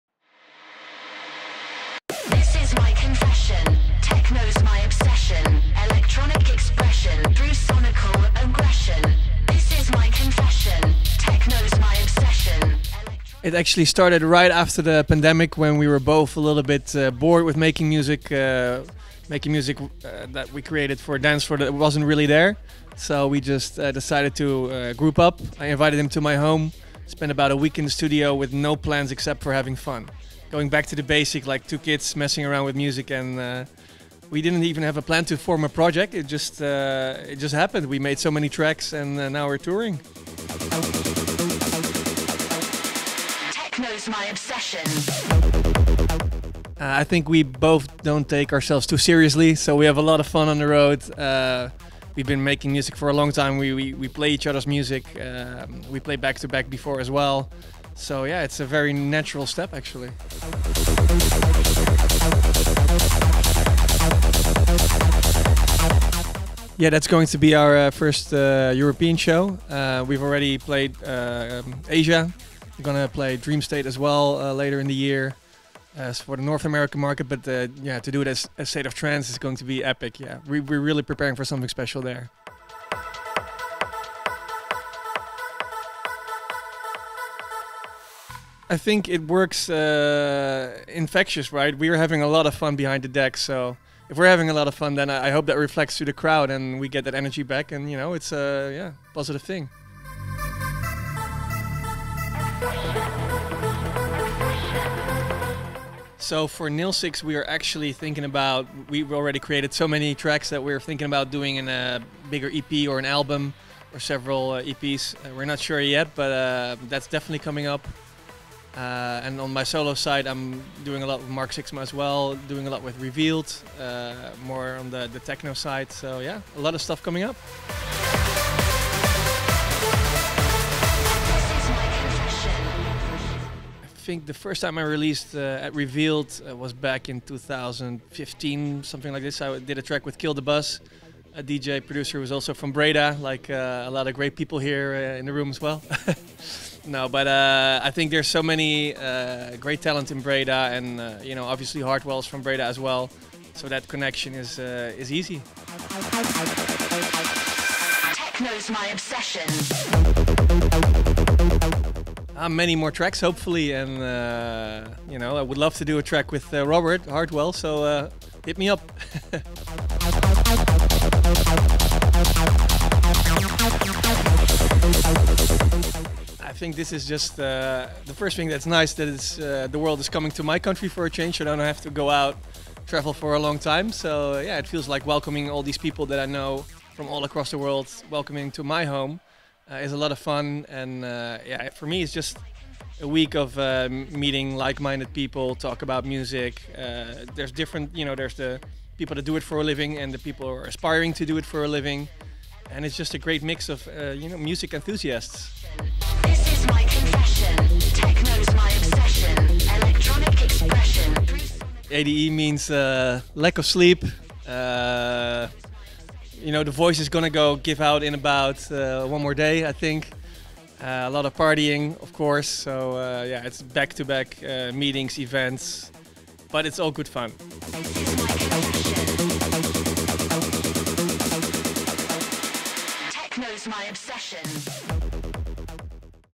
Mark Sixma: I would love to do a track with Hardwell! | Mark Sixma x We Rave You Interview
Follow Mark Sixma